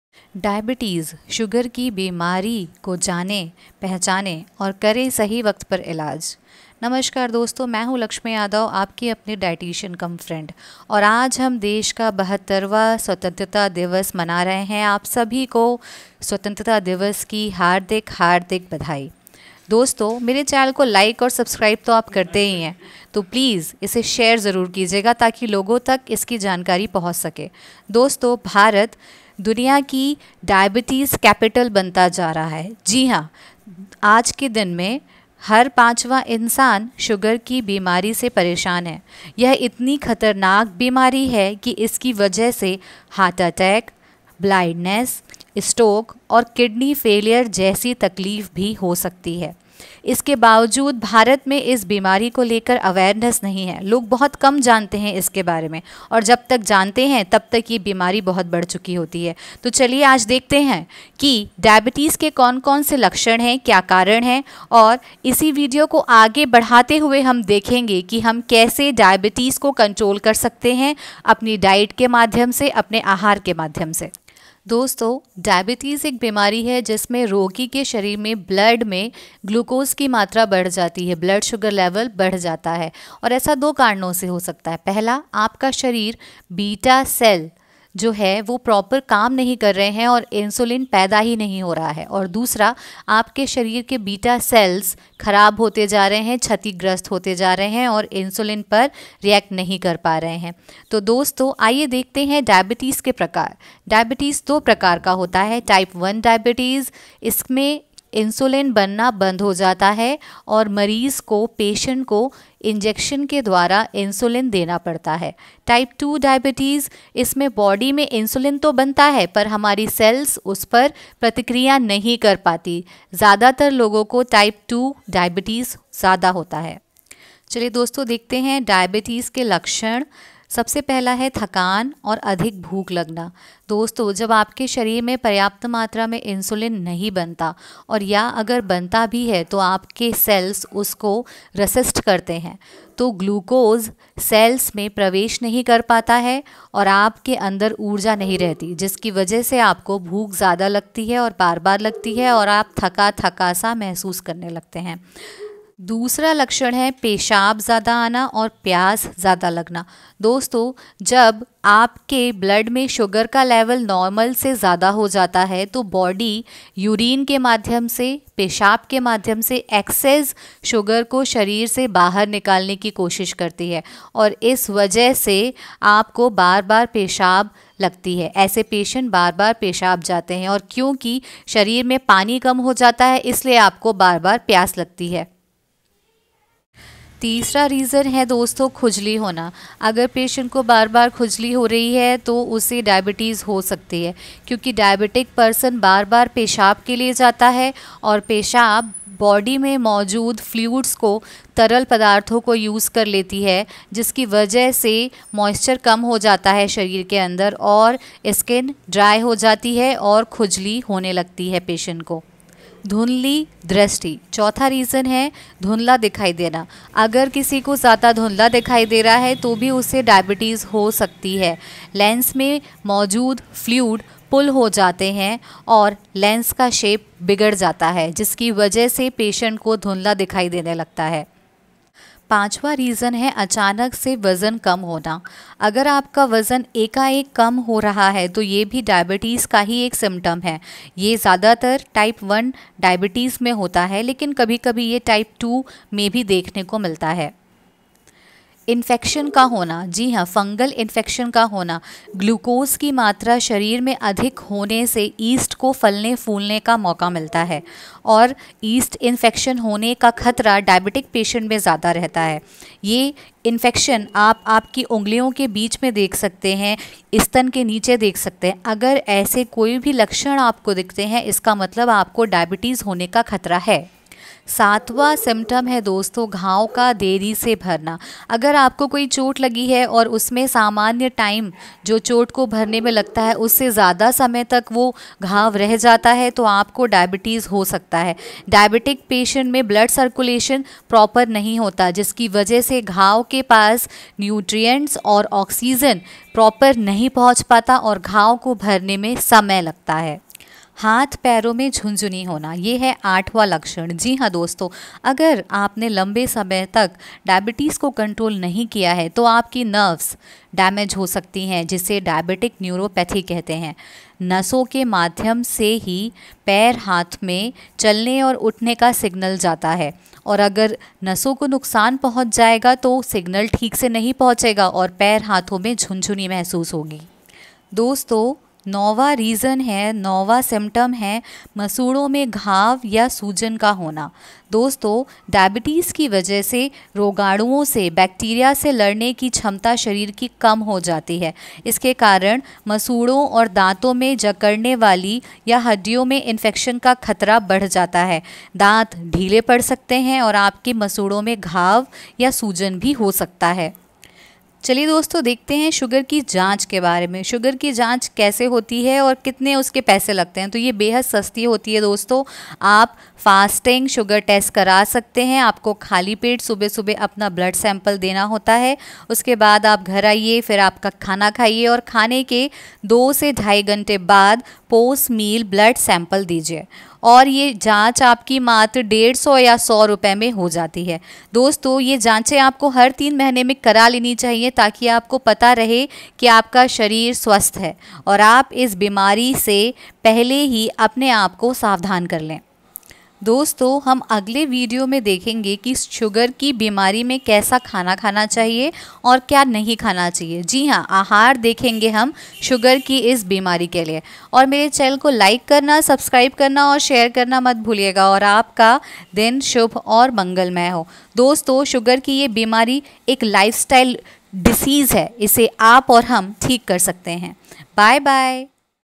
शुगर/ Diabetes की बीमारी के लक्षण... करें सही वक़्त पर ईलाज/symptoms of diabetes/diet in diabetes
Symptoms of Diabetes